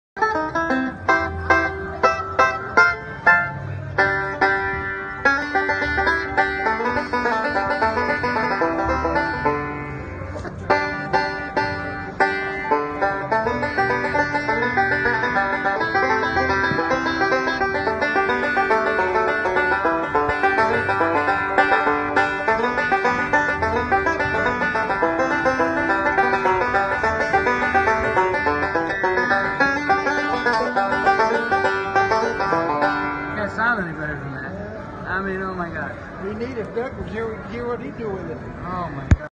greg cahill 1
Greg Cahill, Playing My Gibson Granada 9562-6 with  original tone Ring